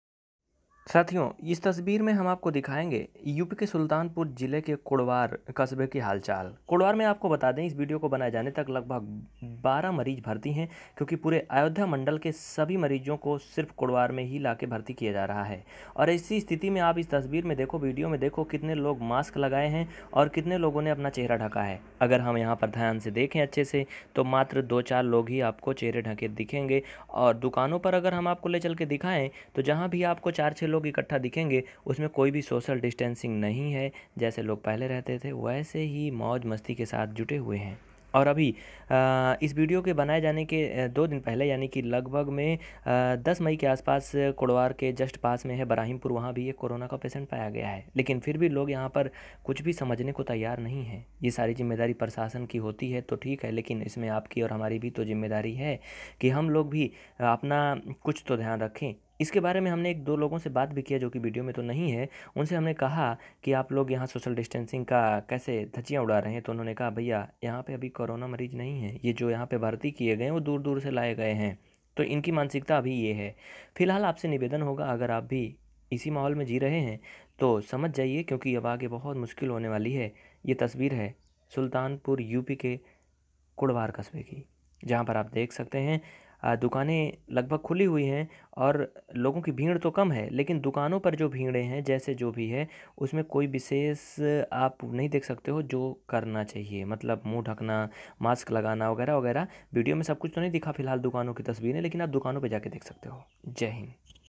पूरे अयोध्या मंडल के Co-na मरीज भर्ती हैं यहां,लेकिन यहां के लोगों में कोई खौफ नहीं
Kudwar news,up news,Sultanpur News in Hindi, sultanpur samachar, Sultanpur Khabar, hindi news,